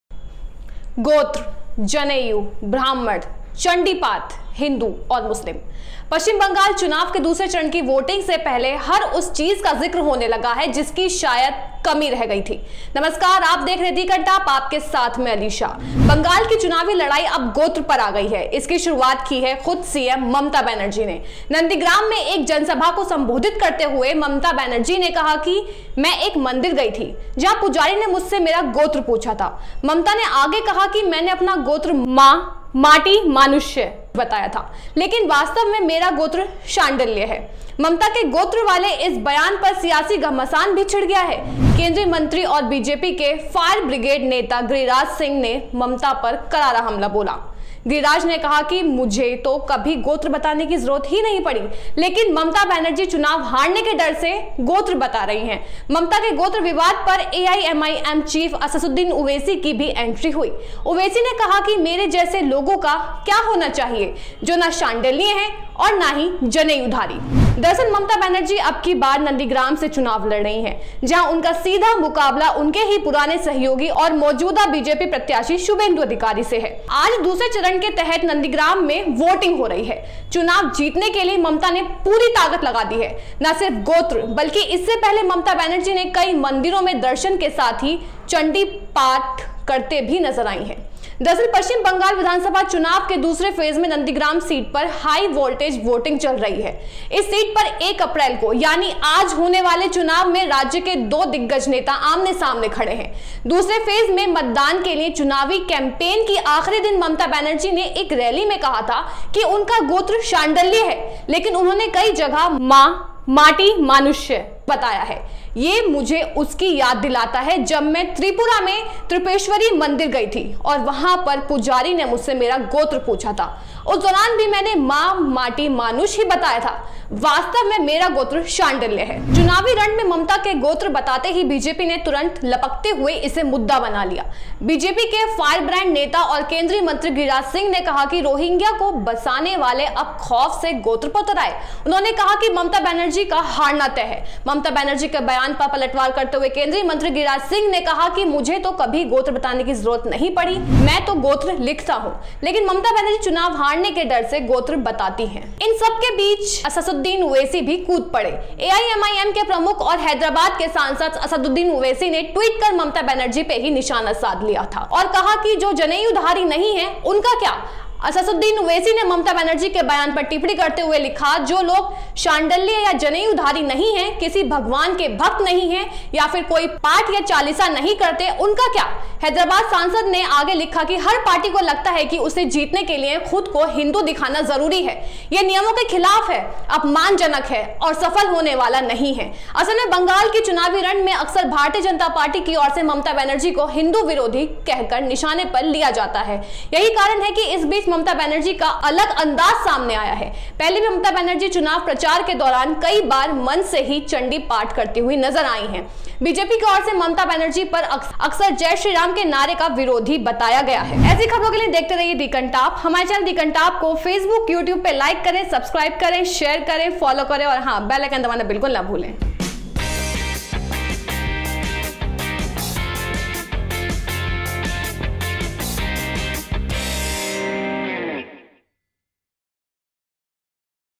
गोत्र पर छिड़ी जंग में कौन होगा किस पर भारी
बंगाल की चुनावी लड़ाई अब गोत्र पर आ गई है... इसकी शुरुआत की है खुद सीएम ममता बनर्जी ने... नंदीग्राम में एक जनसभा को संबोधित करते हुए ममता बनर्जी ने कहा कि मैं एक मंदिर गईं थी... जहां पुजारी ने मुझसे मेरा गोत्र पूछा था... ममता ने आगे कहा कि, मैंने अपना गोत्र 'मां, माटी, मानुष' बताया था... लेकिन, वास्तव में मेरा गोत्र शांडिल्य हैं... ममता के गोत्र वाले इस बयान पर सियासी घमासान भी छिड़ गया... केन्द्रीय मंत्री और बीजेपी के फायरब्रांड नेता गिरिराज सिंह ने ममता पर करारा हमला बोला... गिरिराज ने कहा कि मुझे तो कभी गोत्र बताने की जरूरत नहीं पड़ी... लेकिन, ममता बनर्जी, चुनाव हारने के डर से गोत्र बता रही हैं... ममता के गोत्र विवाद पर AIMIM चीफ असदुद्दीन ओवैसी की भी एंट्री हुई... ओवैसी ने कहा कि मेरे जैसे लोगों का क्या होना चाहिए जो ना शांडिल्य हैं और ना ही जनेऊधारी...

समझे...